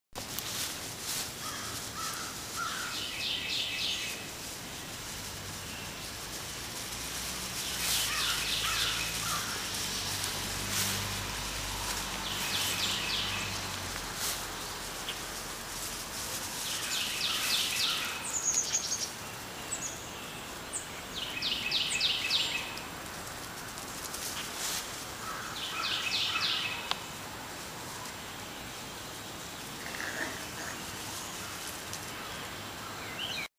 Rocket and Bourre the Schipperke Puppies
Cute puppies playing